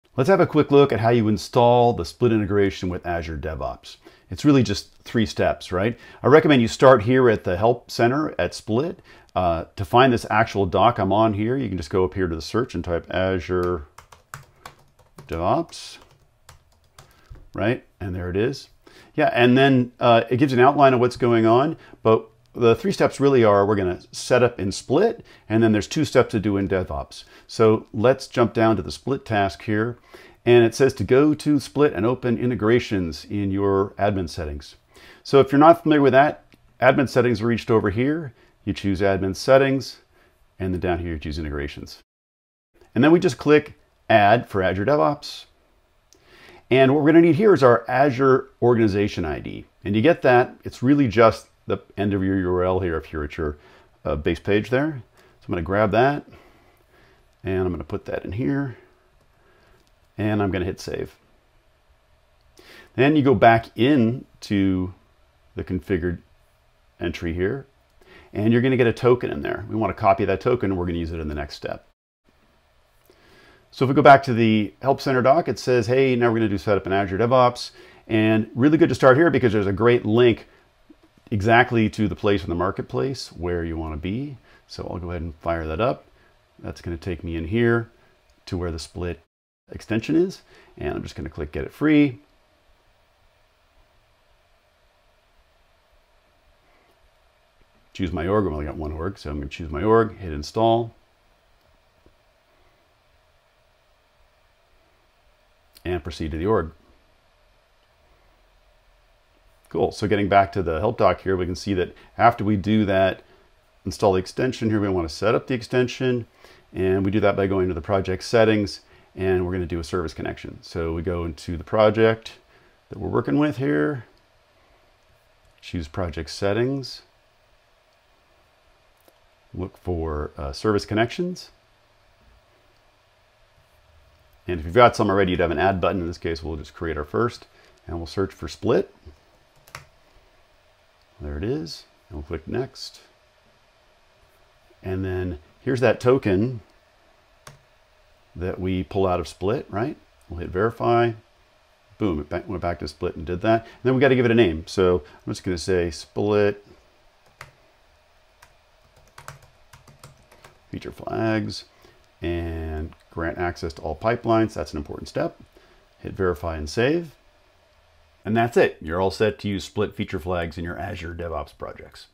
Install Split Integration With Azure DevOps
Here are the three steps for installing the Split integration with Azure DevOps. Need help with a specific step? Jump here:
0:00 Start at Split's Help Center
0:31 ONE: Add Azure DevOps in Admin Settings, Integrations in Split
1:15 TWO: Install Split from the Azure Marketplace
1:50 THREE: Add Split as a new Service Connection in Azure DevOps Project Settings
2:43 Important: Be sure to Grant access to all pipelines during step three.

Want to see what it's like to use Split and Azure DevOps together? Here you go

✨ Thanks for checking out the Split Software YouTube channel!